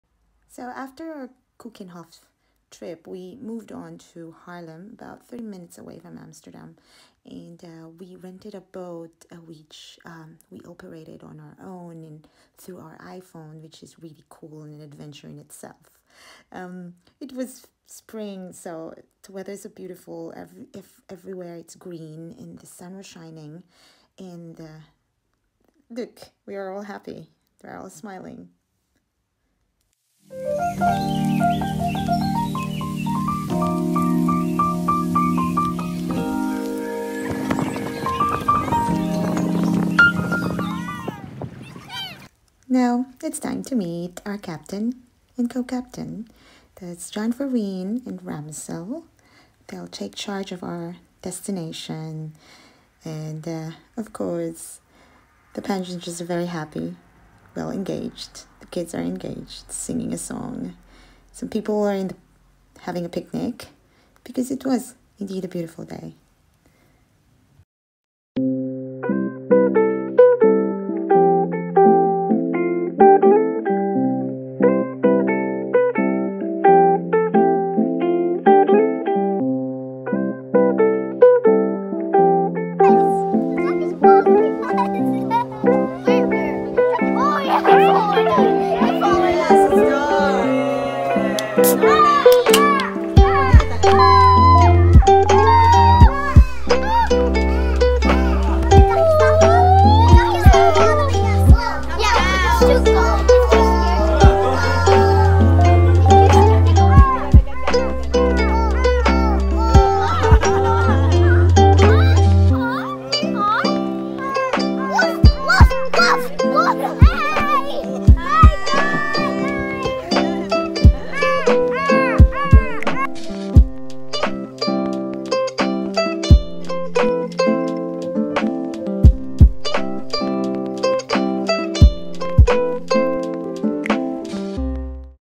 One Spring day in Haarlem
Haarlem is approximately 30 minutes commute from Amsterdam, it’s quieter and less touristy and that’s what i love most!

We rented a boat from this company, sloepdelen that can accommodate 12 persons.  A mix of Sunshine, drizzles and winds that day and we had the time of our lives!

Music Credits to Inshot